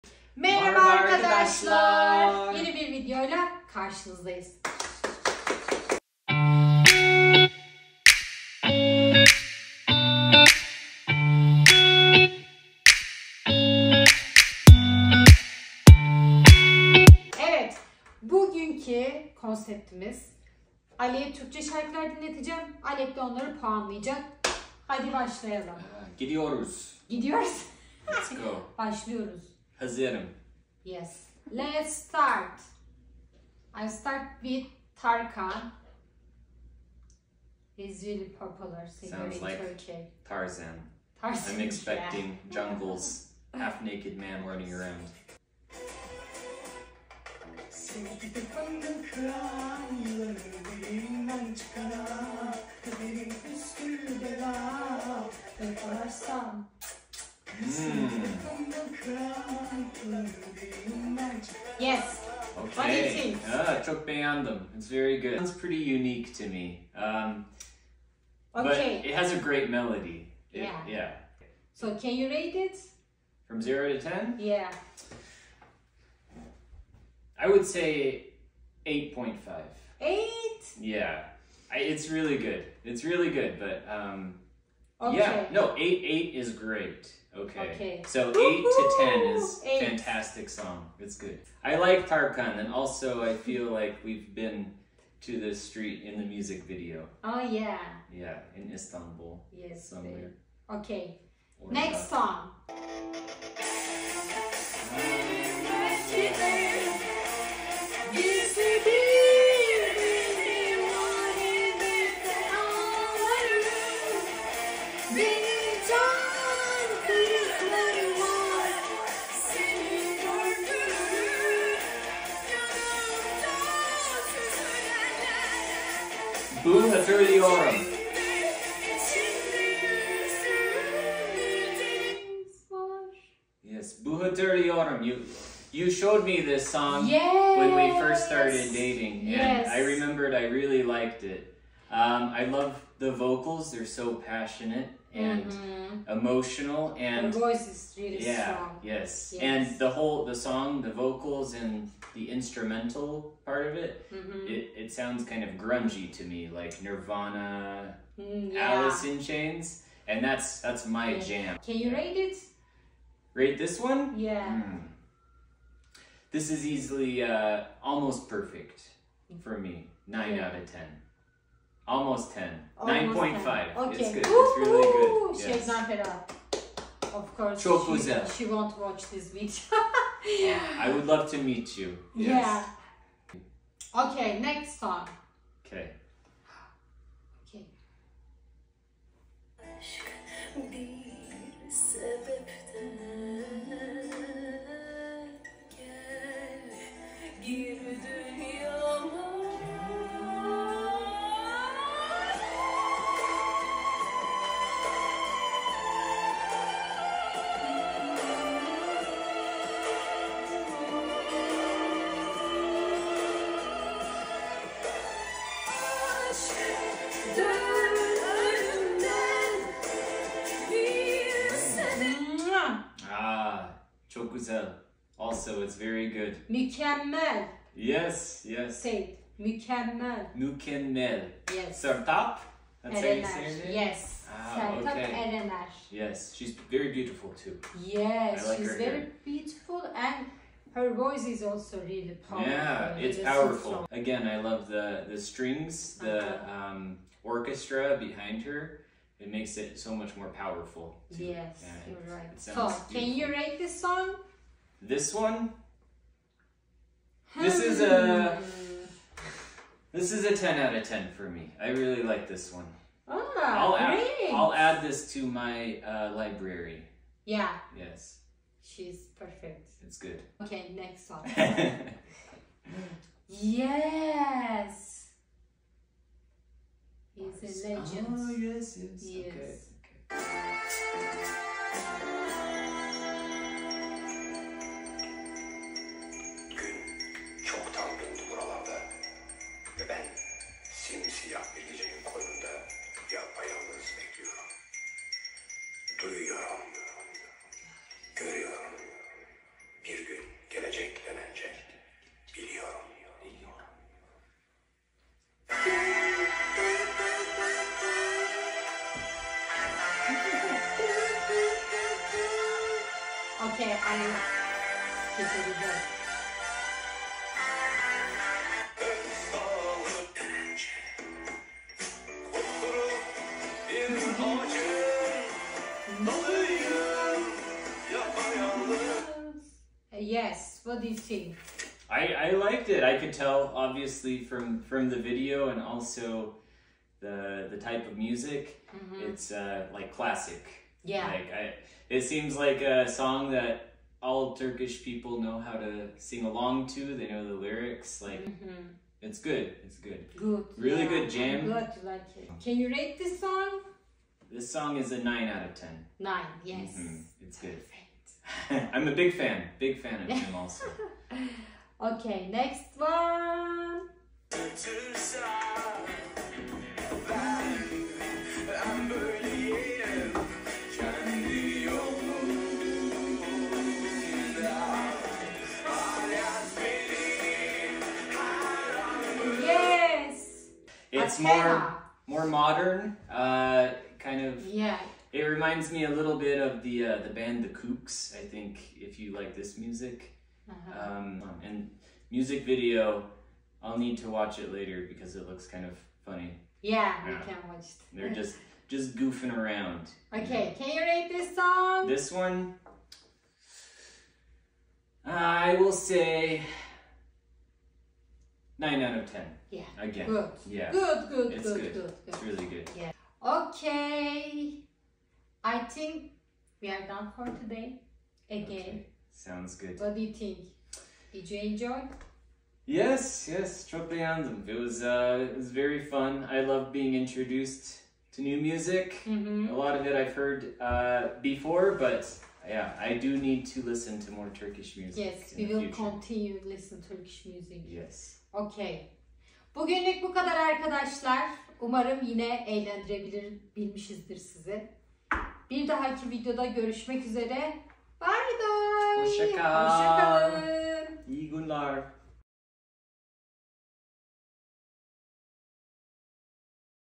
Amerikalı eşim Türk şarkılarını puanlıyor | My American husband rates Turkish songs
Türkçe çeviri için altyazıyı açmayı unutmayın :)